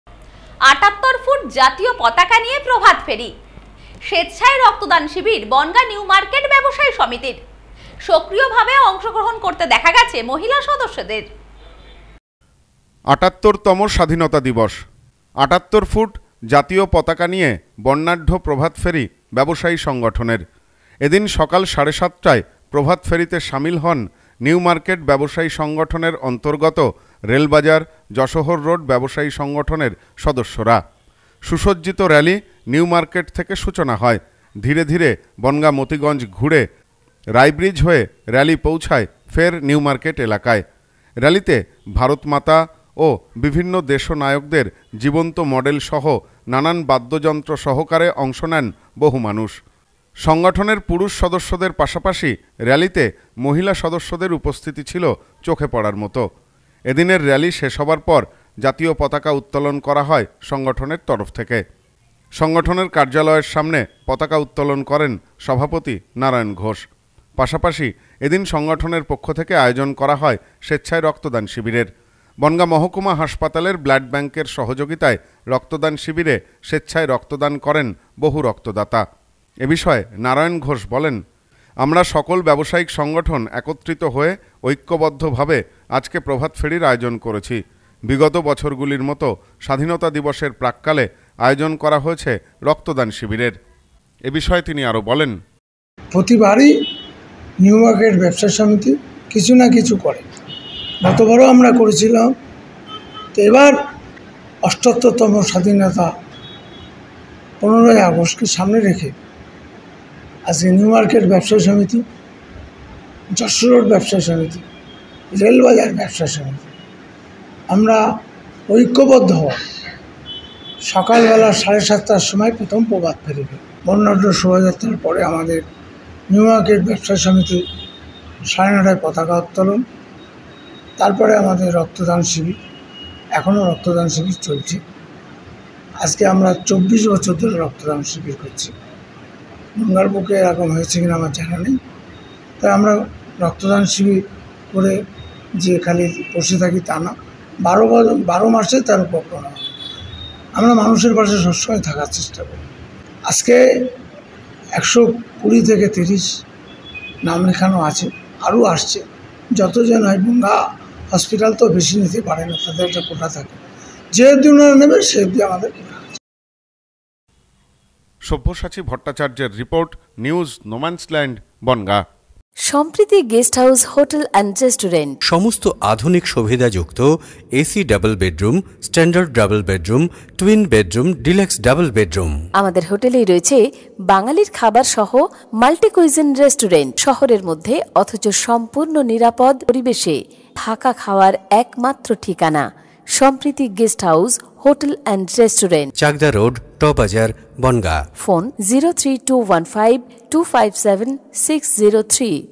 ৭৮ ফুট জাতীয় পতাকা নিয়ে প্রভাত ফেরী।
৭৮ ফুট জাতীয় পতাকা নিয়ে প্রভাত ফেরী। স্বেচ্ছায় রক্তদান শিবির বনগাঁ নিউমার্কেট ব্যবসায়ী সমিতির। সক্রিয় অংশগ্রহণ মহিলা সদস্যদের।